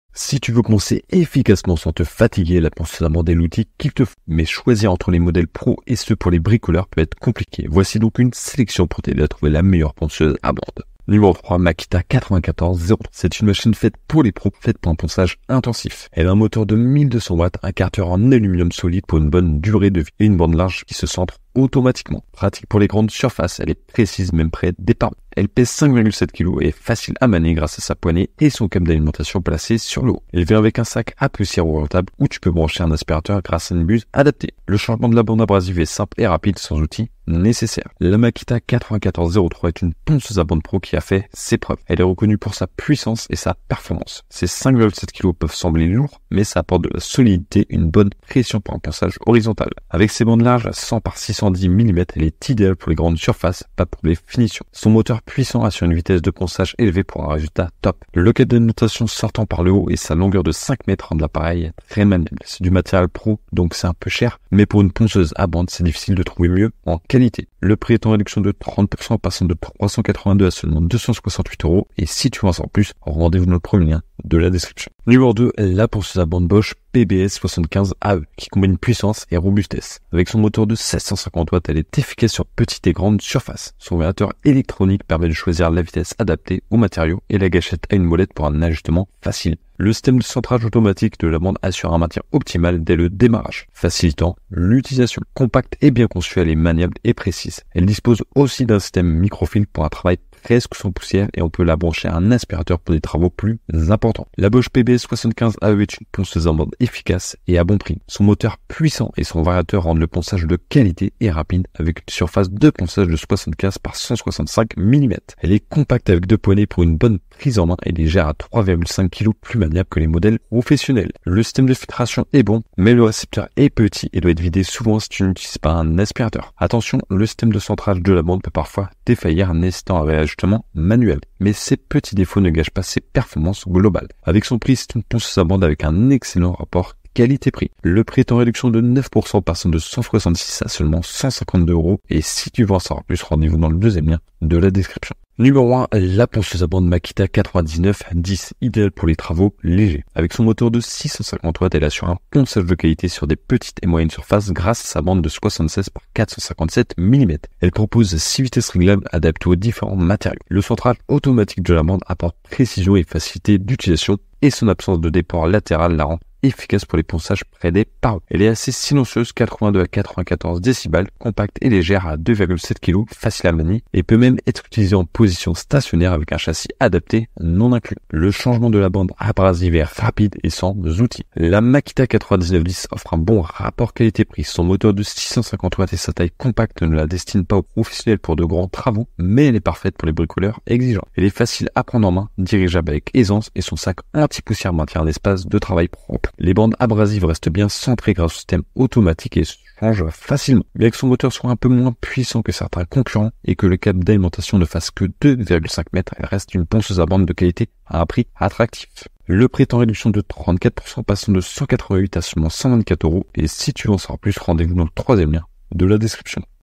Les 3 (vraies) meilleures ponceuses à bande en 2024
🥉 Makita 9403
🥈 Bosch PBS 75 AE
🥇 Makita 9910

Si tu veux faire des travaux chez toi, pense à la ponceuse à bande. C'est super pratique pour poncer sans te fatiguer. Elle a des bandes abrasives pour travailler sur le bois ou le métal, selon le modèle.

Que ce soit pour décaper un parquet, retaper un escalier, ou préparer des volets à peindre, elle est vraiment utile, même pour les débutants en bricolage. Tu peux l'utiliser fixe ou en bougeant, mais fais gaffe à la sécurité pour éviter les accidents.

Un truc cool, c'est qu'elle aspire la poussière. C'est presque indispensable vu la quantité qu'elle produit. Avec une ponceuse à bande, ton espace de travail reste propre et c'est mieux pour respirer.

00:00 - Pour poncer plus efficacement, et avec moins d’efforts !
00:11 - Professionnelle
1:15 - Bon rapport qualité / prix
2:25 - Bon rapport qualité / prix 2

Mes vidéos incluent des liens publicitaires. L'utilisation de ces liens ne génère aucun frais supplémentaire pour vous, mais m'octroie une petite commission.